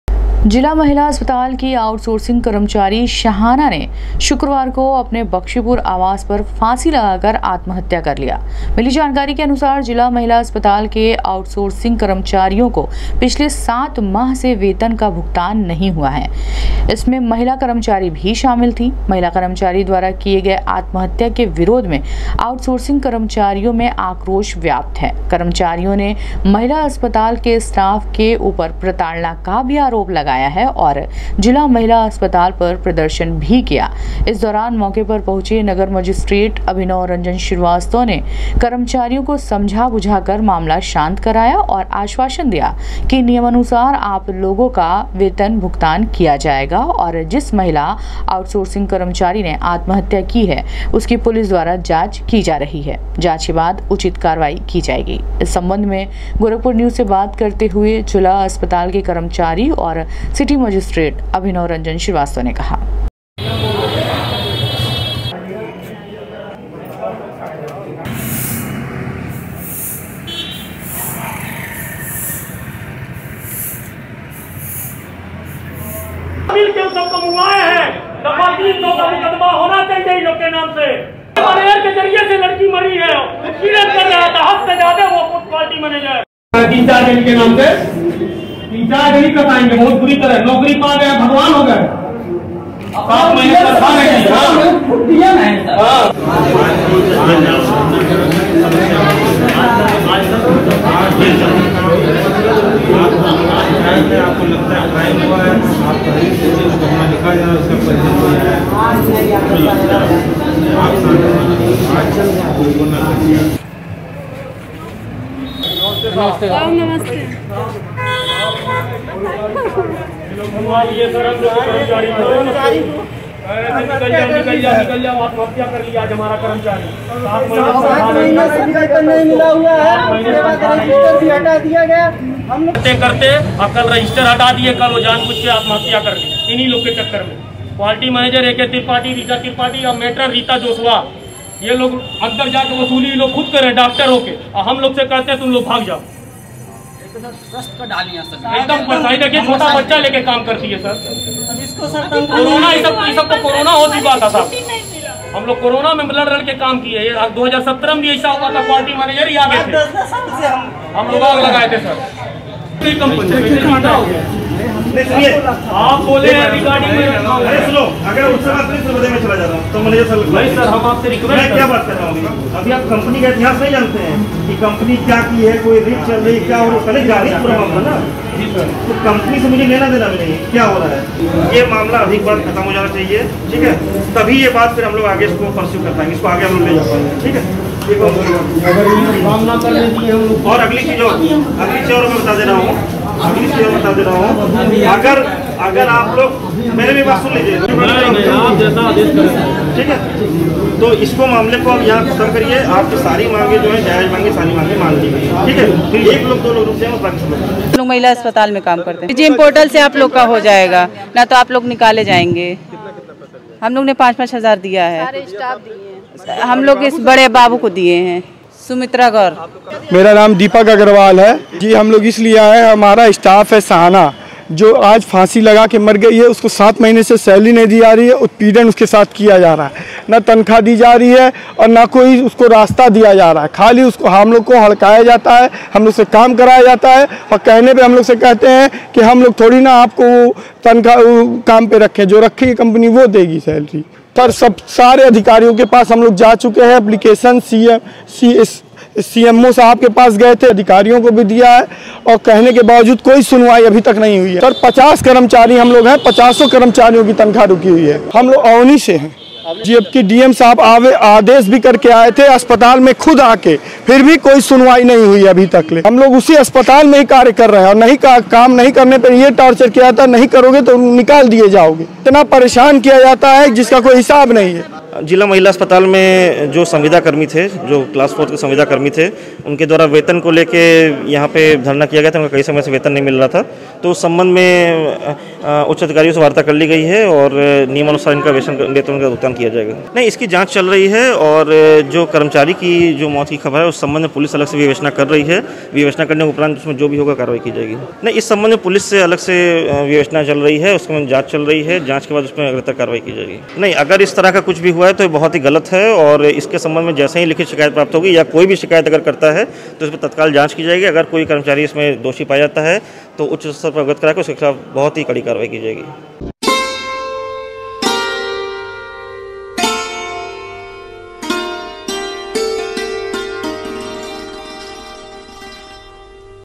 कोतवाली थाना क्षेत्र के बक्सीपुर में जिला महिला अस्पताल की आउट सोर्सिंग महिला कर्मचारी ने की
कोतवाली थाना क्षेत्र के बक्सीपुर में जिला महिला अस्पताल की आउट सोर्सिंग महिला कर्मचारी ने की आत्महत्या ,07 माह से वेतन न मिलने का जिला महिला अस्पताल की महिला कर्मचारी ने लगाया था आरोप